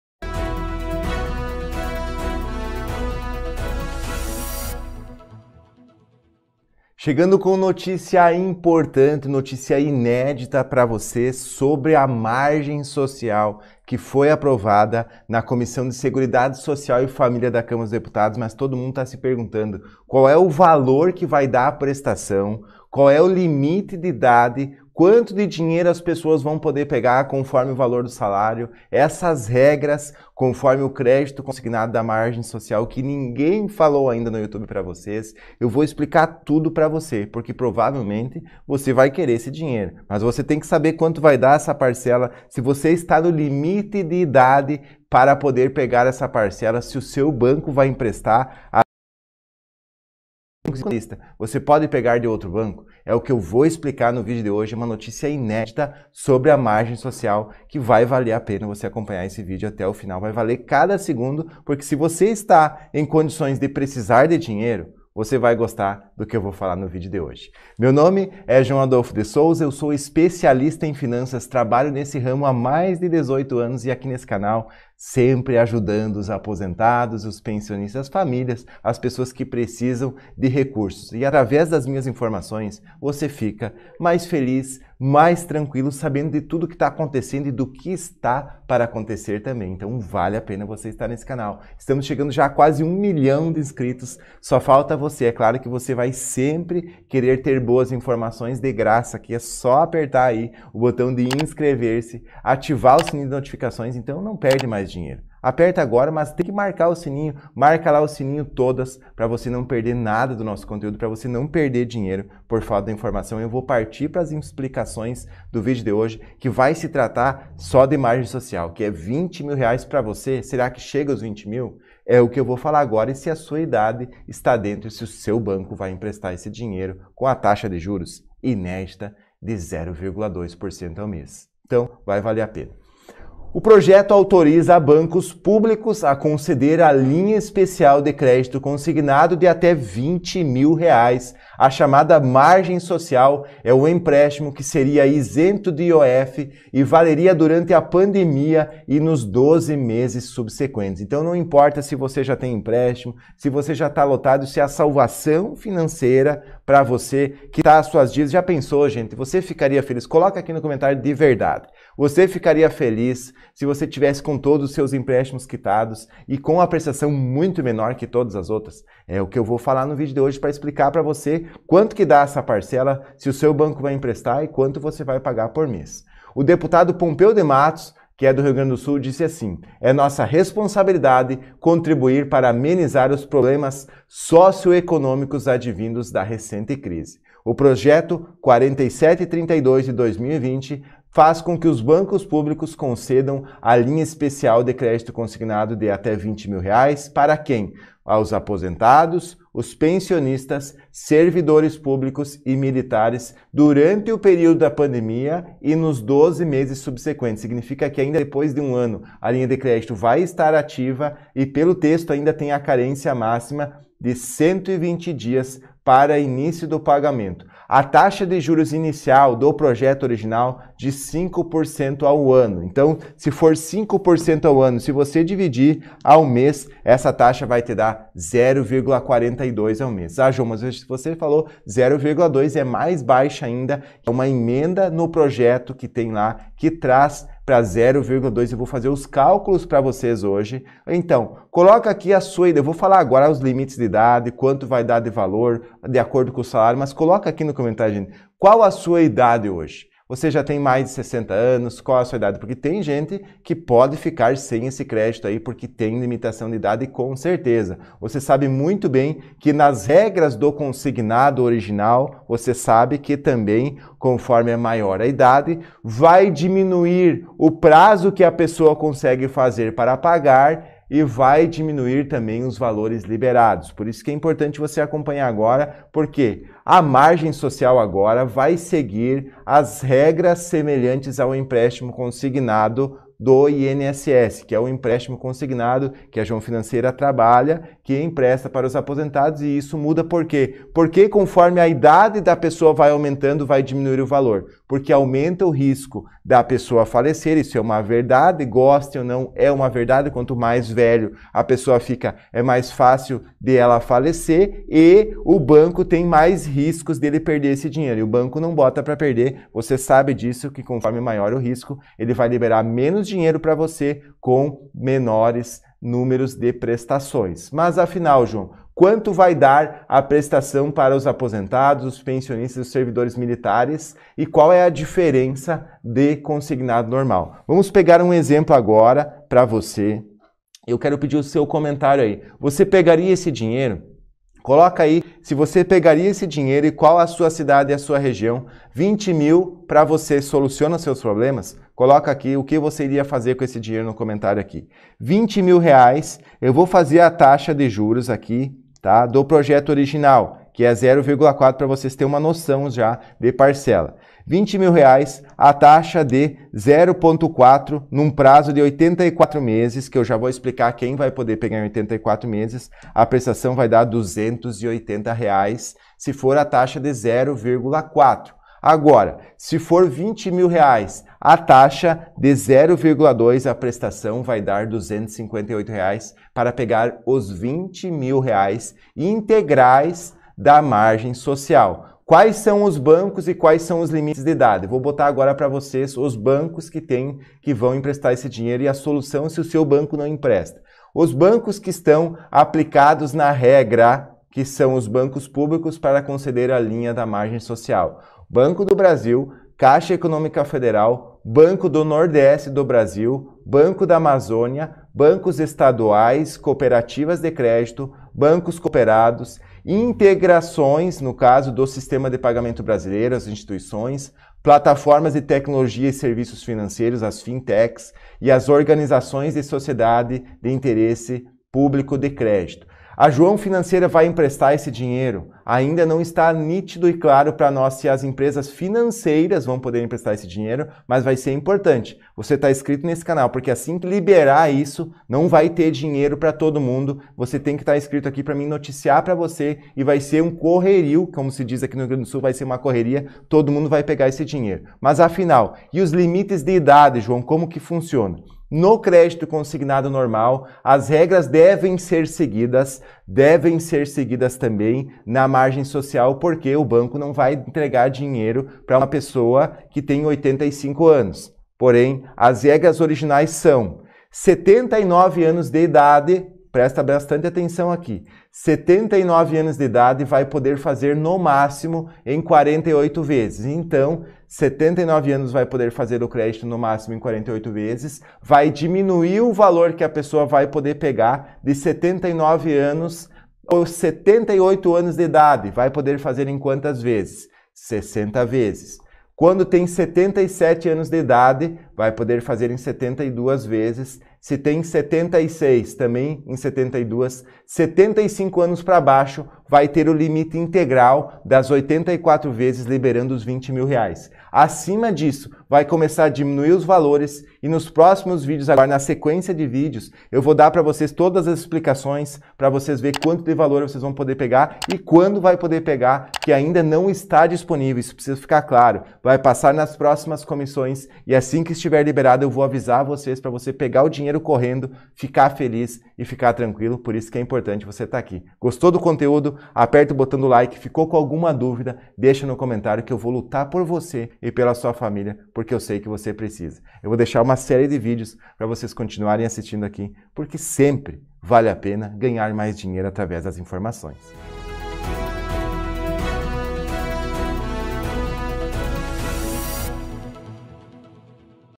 VITÓRIA: MARGEM SOCIAL R$20.000 para TODOS OS APOSENTADOS E PENSIONISTAS DO INSS! SAIU LISTA BANCOS!
NÃO PERCA TEMPO: ACESSE AGORA! VEJA seus DIREITOS e as ÚLTIMAS NOVIDADES do INSS